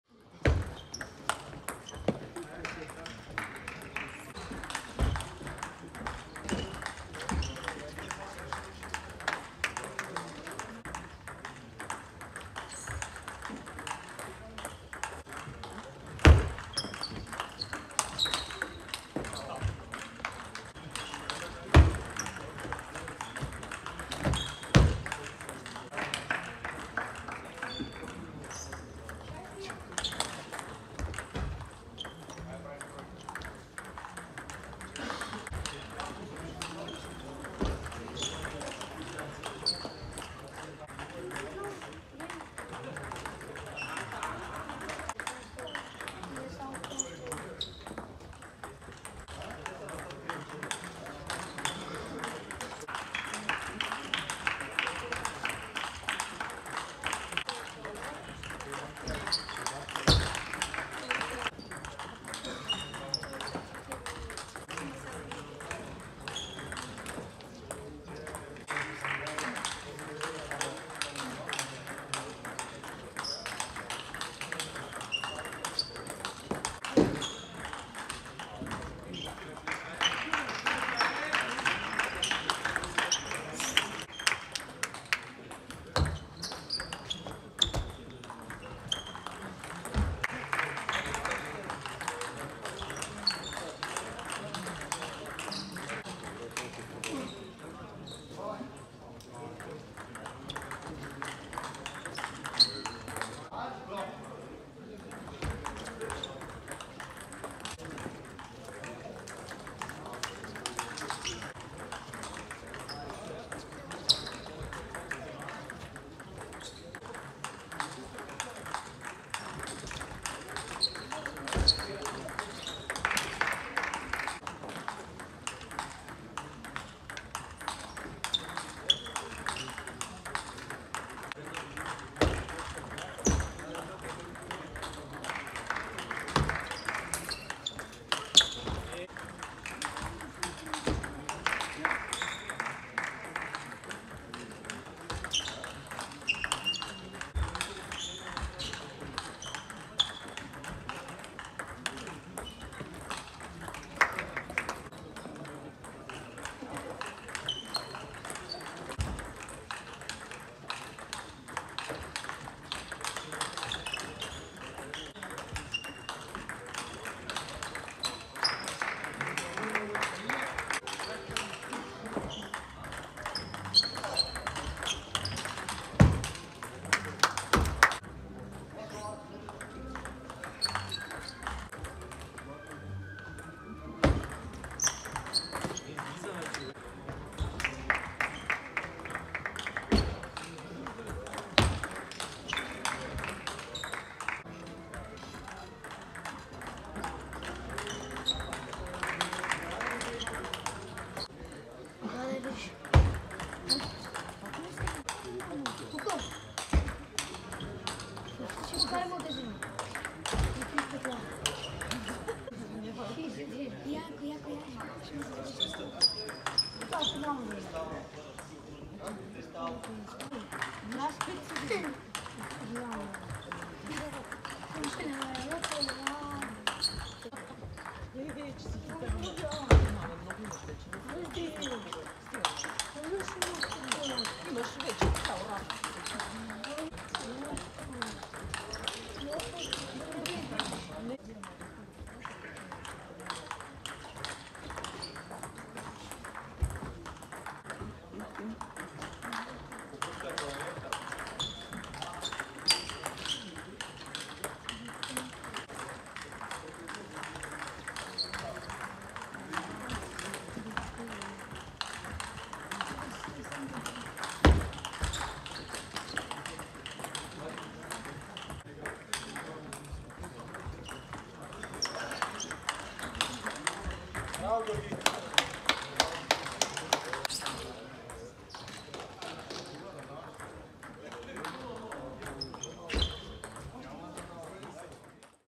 P. KRASTEV (black) vs. Georgi GOGOV (blue) | Season 2019/2020 | 25-26.10.2019
Bulgarian Table Tennis Premier League - Men's Team

Players - Petyo KRASTEV vs. Georgi GOGOV
Teams - TTC DIGESTA DUPNITSA vs. TTC METALIC PERNIK

City - VARNA
Tournament - 1st.

Please visit our online store if you need professional table tennis equipment.